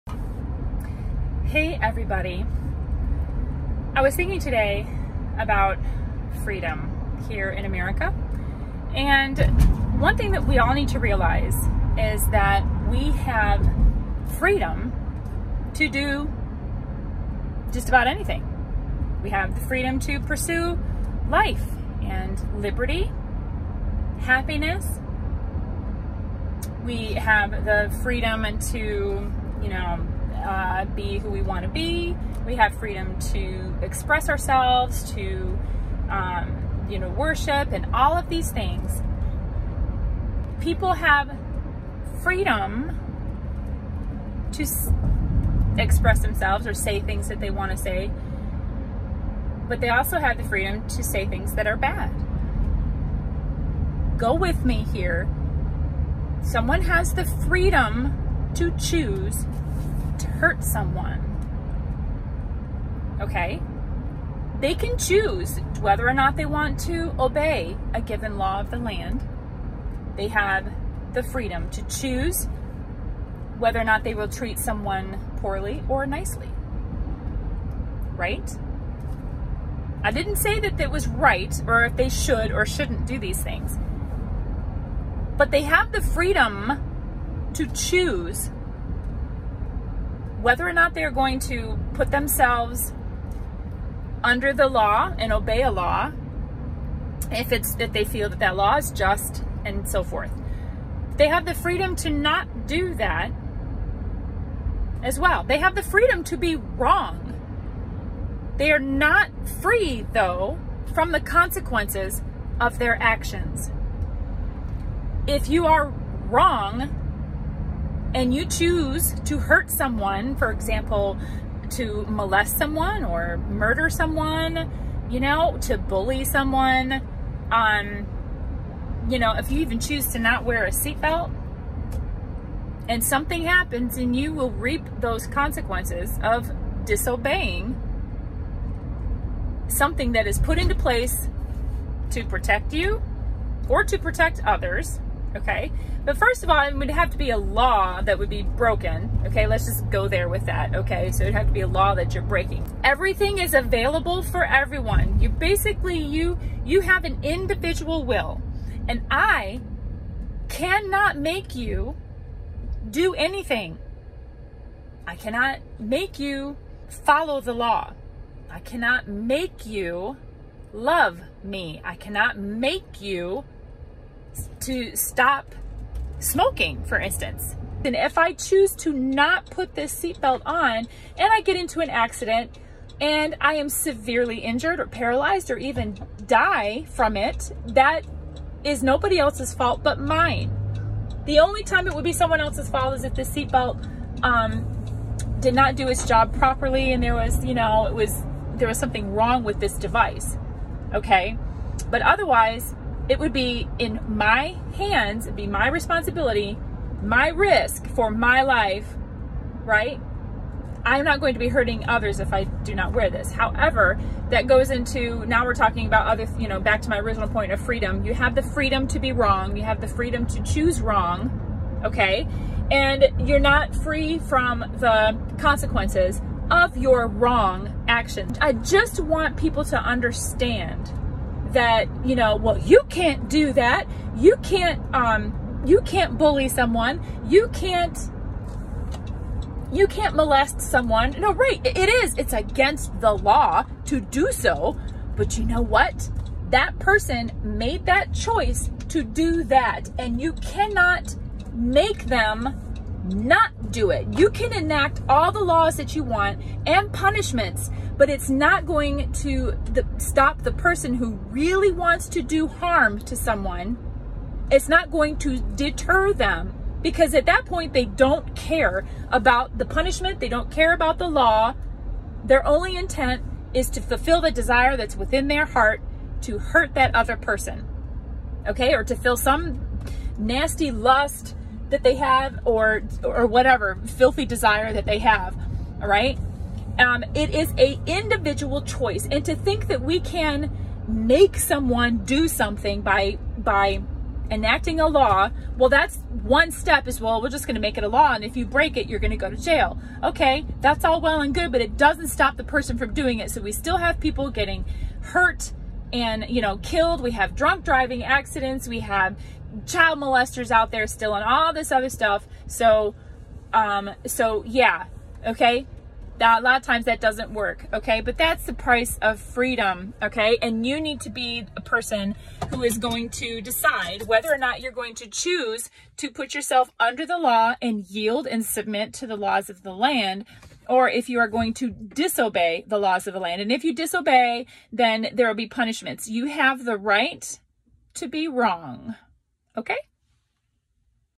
You Have the Right to be Wrong | Freedom in America
Join Darlene in her car as she discusses free will and consequences.